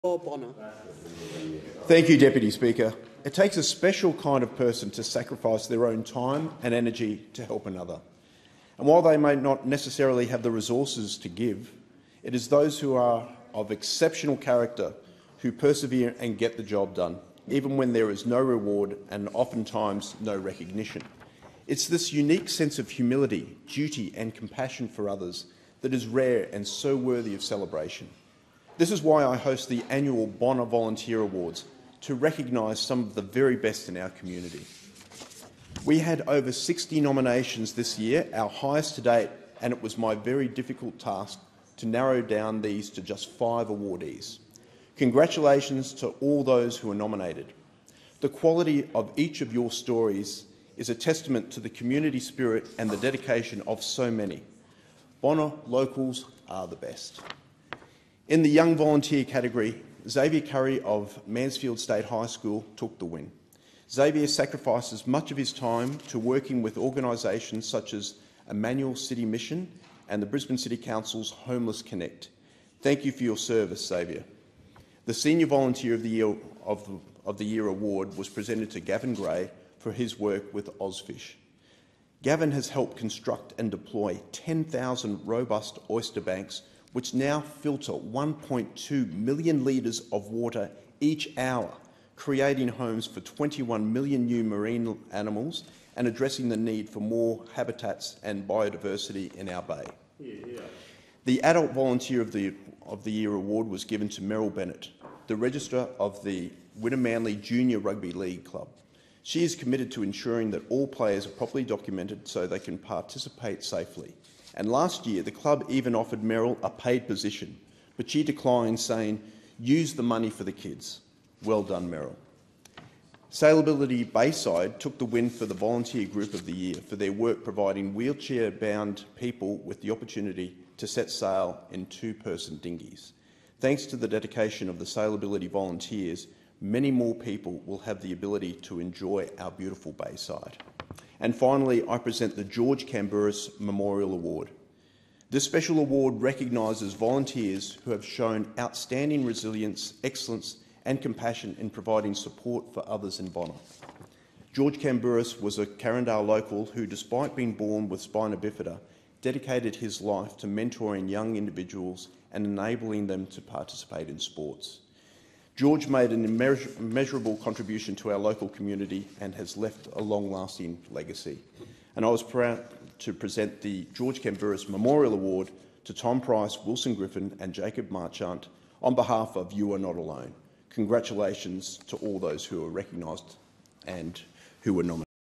Bonner Volunteer Awards: Congratulation!
It takes special kind of person to sacrifice their own time, and energy to help another.

While they may not necessarily have the resources or effort to give, it is those who are of exceptional character who persevere and get the job done – even when there is no reward or often times recognition.
It’s this unique sense humility, duty, and compassion for others, that is rare and so worthy of celebration.

This is why I host the annual Bonner Volunteer Awards – to recognise some of the very best in our community.

We had over 60 nominations this year, our highest to date, and it was my difficult task to narrow down these to just 5 awardees!

Congratulations to all those who were nominated.
The quality of each of your stories is a testament to the community mindedness and dedication of so many….
Bonner locals are the Best!
In the Young Volunteer Category, Xavier Curry, of Mansfield State High School, took the win!

Xavier sacrifices much of his time to working with organisations such as the Emmanuel City Mission and Brisbane City Council’s Homeless Connect.
Thank you for your service, Xavier!

The Senior Volunteer of the Year Award was presented to Gavin Gray, for his work with OzFish.
Gavin has helped construct and deploy 10,000 Robust Oyster Banks which now filter 1.2 million litres of water every hour… creating homes for 21 million new marine animals and addressing the need for more habitats and biodiversity in our bay.
The Adult Volunteer of the Year Award was given to Merryl Bennett, the registrar at the Wynnum Manly Junior Rugby League Club.

She is committed to ensuring all players are properly documented, so they can participate safely.

Last year the Club even offered Meryl a paid position, but she declined saying “use the money for the kids.”
Sailability Bayside took the win for Volunteer Group of the Year, for their work providing wheelchair bound people with the opportunity to set sail in two person dinghies!

Thanks to the dedication of the Sailaibility volunteers, many more people have been able to enjoy our beautiful bayside.

Finally, I presented the George Kambouris Memorial Award.

This special award recognises volunteers who have shown outstanding resilience, excellence, and compassion in providing support for others in Bonner.

George Kambouris was a Carindale local who, despite being born with spina bifida, dedicated his life to mentoring young, individuals and enabling them to participate in sports.

George made an immeasurable contribution to our local community and has left a long-lasting legacy.

I was proud to present the George Kambouris Memorial Award to Tom Price, Wilson Griffin, and Jacob Marchant on behalf of You Are Not Alone.

Congratulations to all those who were nominated and recognised!